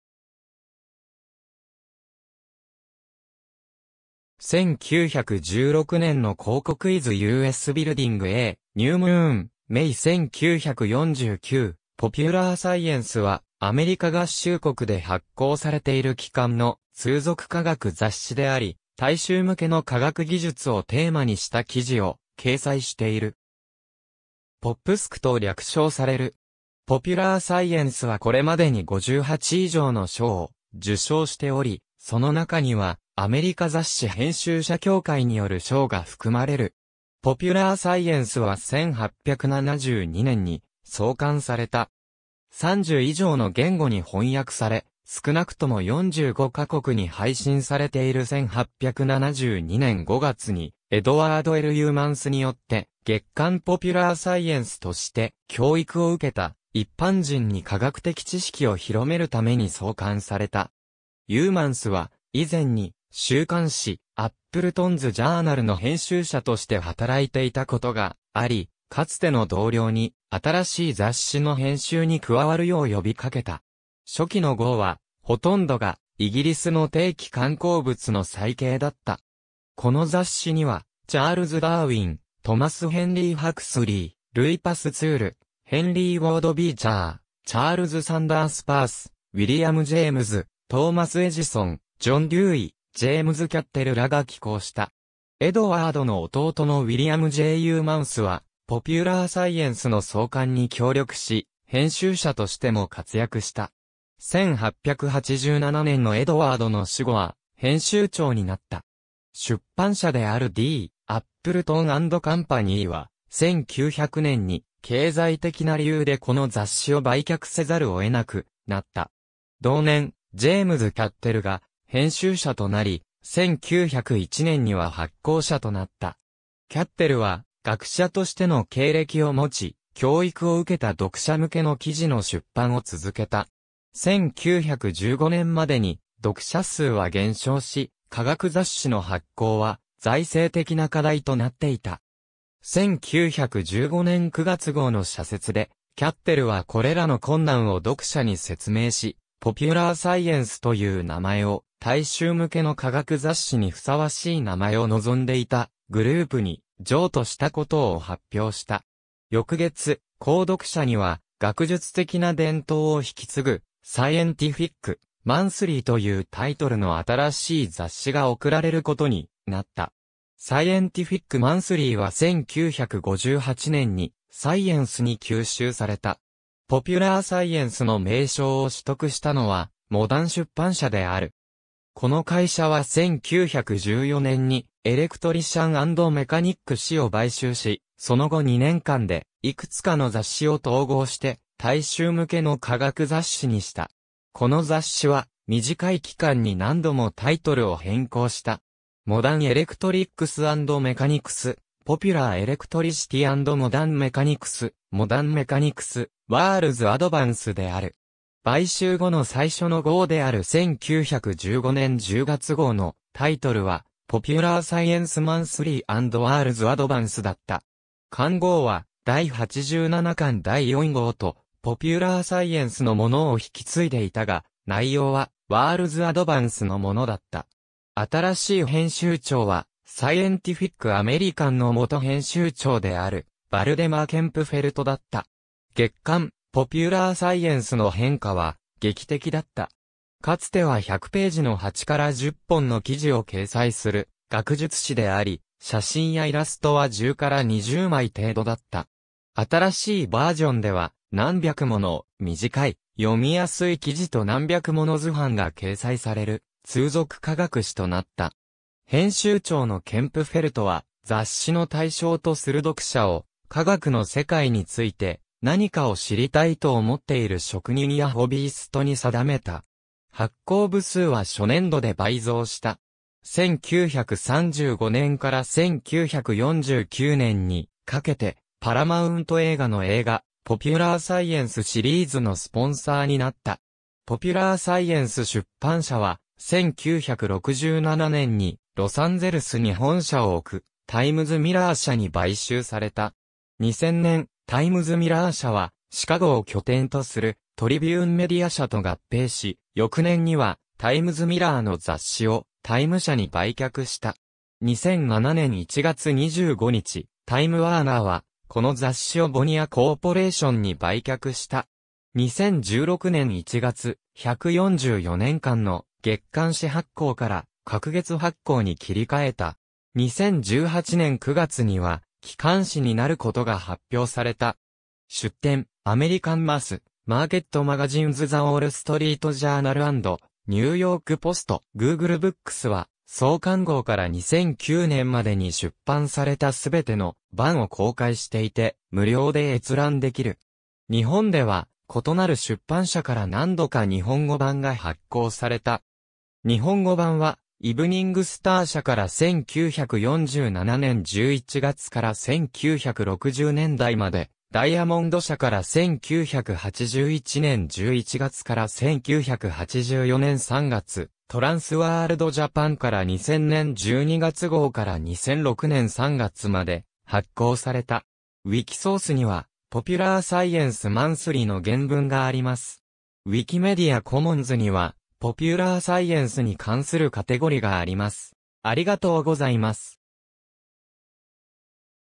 ポピュラーサイエンス
タイム_(出版社)
1916年の広告 Is U.S Building a "New Moon"?, May 1949 『ポピュラーサイエンス』（Popular Science）は、アメリカ合衆国で発行されている季刊の通俗科学雑誌であり、大衆向けの科学技術をテーマにした記事を掲載している。
PopSciと略称される。
『ポピュラーサイエンス』はこれまでに58以上の賞を受賞しており、その中には、アメリカ雑誌編集者協会による賞（2003年の一般優秀賞、2004年の雑誌部門最優秀賞、2019年の単発特集号）が含まれる。
『ポピュラーサイエンス』は1872年に創刊された。
30以上の言語に翻訳され、少なくとも45カ国に配信されている 1872年5月にエドワード・L・ユーマンスによって、『月刊ポピュラーサイエンス』(Popular Science Monthly)として、教育を受けた一般人に科学的知識を広めるために創刊された。
ユーマンスは以前に週刊誌『アップルトンズ・ジャーナル』(Appleton's Journal)の編集者として働いていたことがあり、かつての同僚に新しい雑誌の編集に加わるよう呼びかけた。
初期の号は、ほとんどがイギリスの定期刊行物の再掲だった。
この雑誌には、チャールズ・ダーウィン、トマス・ヘンリー・ハクスリー、ルイ・パスツール、ヘンリー・ウォード・ビーチャー、チャールズ・サンダース・パース、ウィリアム・ジェームズ、トーマス・エジソン、ジョン・デューイ、ジェームズ・キャッテルらが寄稿した。
エドワードの弟のウィリアム・ジェイ・ユーマンスは、『ポピュラーサイエンス』の創刊に協力し、編集者としても活躍した。
1887年のエドワードの死後は編集長になった 。
出版社であるD・アップルトン・アンド・カンパニーは、1900年に経済的な理由でこの雑誌を売却せざるを得なくなった。
同年、ジェームズ・キャッテルが編集者となり、1901年には発行者となった。
キャッテルは学者としての経歴を持ち、教育を受けた読者向けの記事の出版を続けた。
1915年までに読者数は減少し、科学雑誌の発行は財政的な課題となっていた。
1915年9月号の社説で、キャッテルはこれらの困難を読者に説明し、「ポピュラーサイエンス」という名前を、大衆向けの科学雑誌にふさわしい名前を望んでいたグループに「譲渡」したことを発表した。
翌月、購読者には、学術的な伝統を引き継ぐ『サイエンティフィック・マンスリー』(The Scientific Monthly)というタイトルの新しい雑誌が送られることになった。
『サイエンティフィック・マンスリー』は1958年に『サイエンス』に吸収された。
「ポピュラーサイエンス」の名称を取得したのはモダン出版社である。
この会社は1914年に『エレクトリシャン・アンド・メカニック』(Electrician and Mechanic)誌を買収し、その後2年間でいくつかの雑誌を統合して、大衆向けの科学雑誌にした。
この雑誌は短い期間に何度もタイトルを変更した。
『モダン・エレクトリックス・アンド・メカニクス』(Modern Electrics and Mechanics)、『ポピュラー・エレクトリシティ・アンド・モダン・メカニクス』(Popular Electricity and Modern Mechanics)、『モダン・メカニクス』(Modern Mechanics)、『ワールズ・アドバンス』(World's Advance)である。
買収後の最初の号である1915年10月号のタイトルは、『ポピュラー・サイエンス・マンスリー・アンド・ワールズ・アドバンス』(Popular Science Monthly and World's Advance)だった。
巻号は第87巻第4号と『ポピュラーサイエンス』のものを引き継いでいたが、内容は『ワールズ・アドバンス』のものだった。
新しい編集長は『サイエンティフィック・アメリカン』の元編集長であるヴァルデマー・ケンプフェルトだった。
月刊『ポピュラーサイエンス』の変化は劇的だった。
かつては100ページの8～10本の記事を掲載する学術誌であり、写真やイラストは10～20枚程度だった。
新しいバージョンでは、何百もの短い読みやすい記事と何百もの図版が掲載される通俗科学誌となった。
編集長のケンプフェルトは、雑誌の対象とする読者を「科学の世界について何かを知りたいと思っている職人やホビイ...